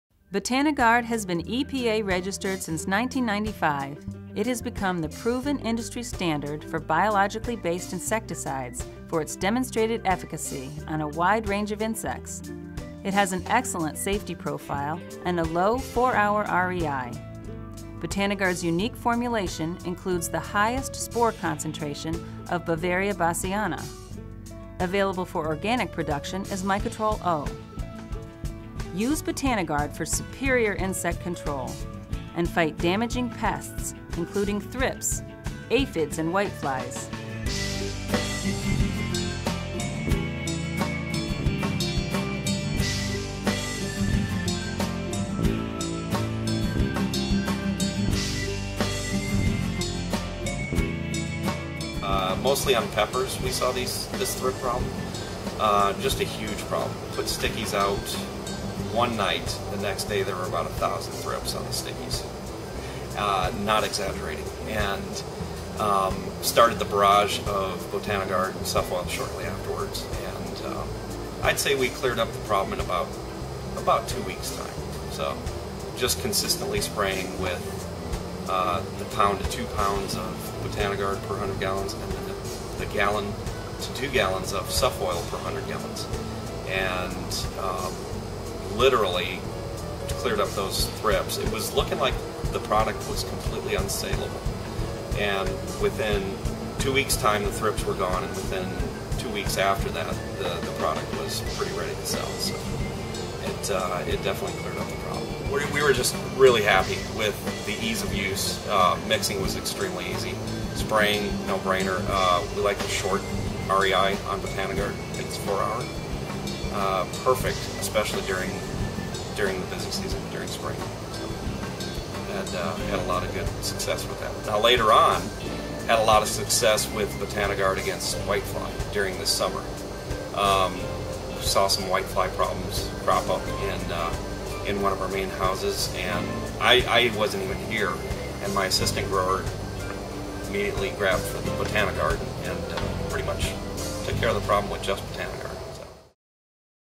BotaniGard - Superior, Biologically-based Insect Control
BotaniGard, from BioWorks has become the proven industry standard for biologically-based insecticides for its demonstrated efficacy against a wide range of pests that attack plants. Unique mode of action, excellent safety profile, OMRI Listed formulation - Mycotrol O for organic growing,